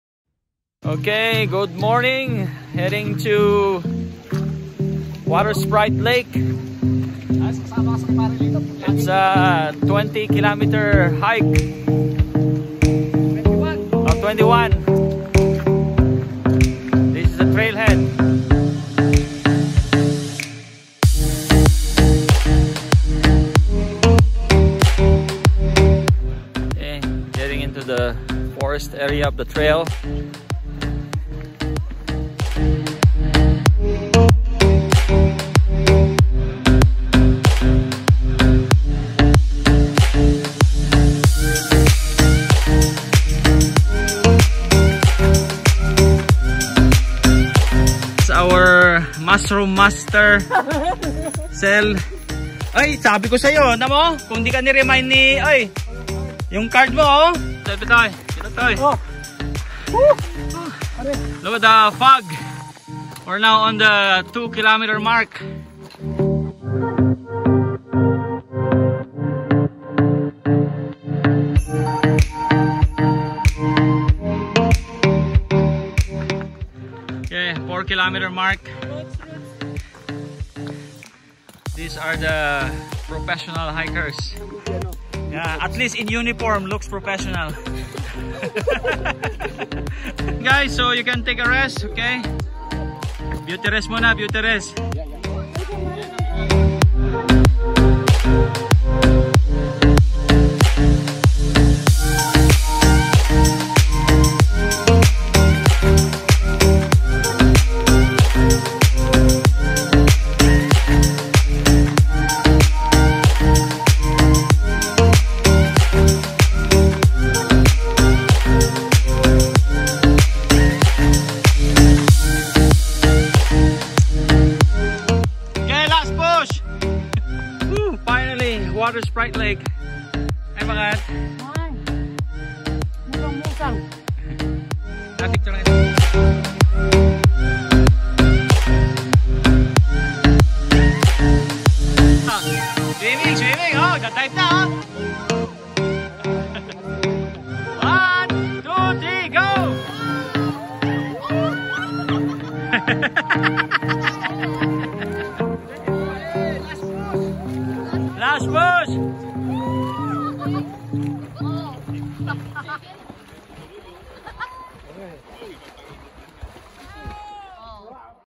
Hike to WaterSprite Lake, Squamish BC
23km hike to this beautiful torquise coloured lake on top of the mountain. It looks us 4.5hrs to get to this place.